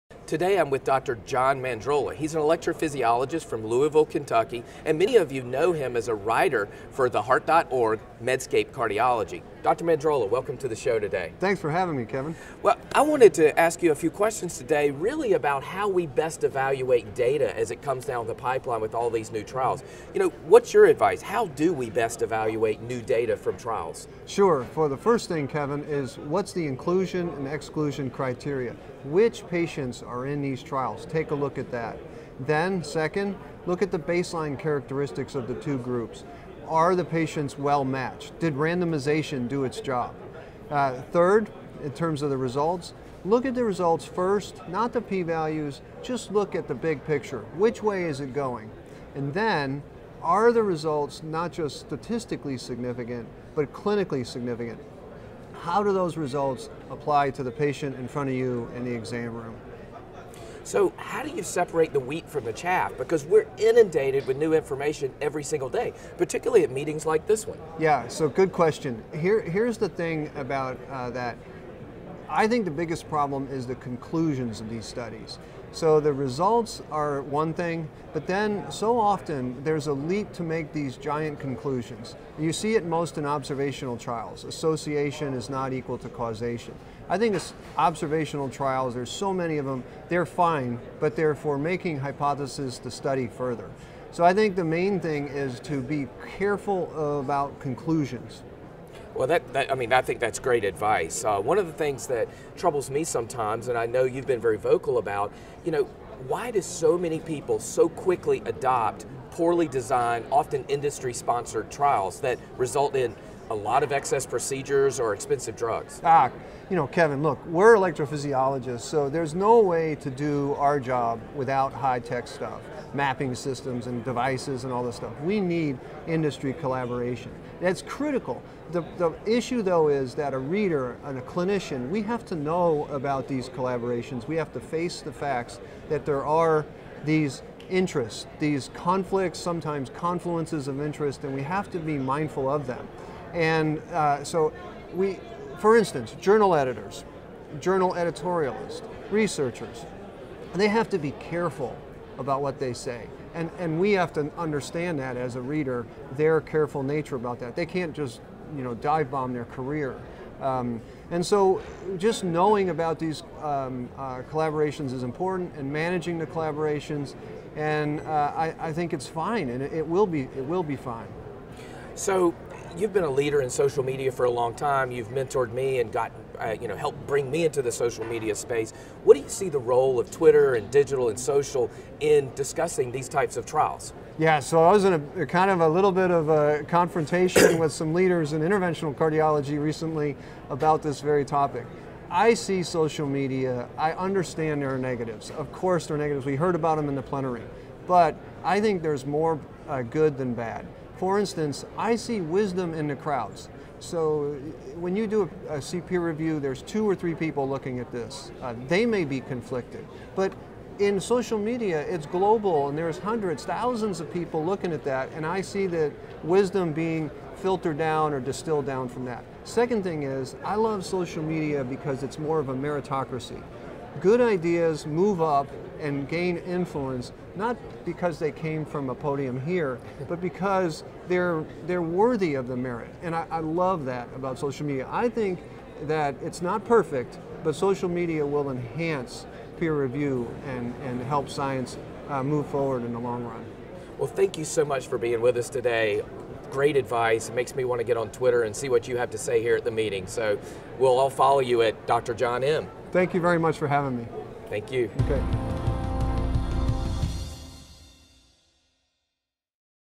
Interview with John Mandrola, MD - HRS 2018
At the 39th annual Heart Rhythm Scientific Sessions in Boston, Heart Rhythm TV is joined in the studio by John Mandrola, MD, a Cardiac Electrophysiologist from Louisville, Kentucky and a writer for the He joins us to talk about how to best evaluate data and how we can use new and innovative technologies to advance the field.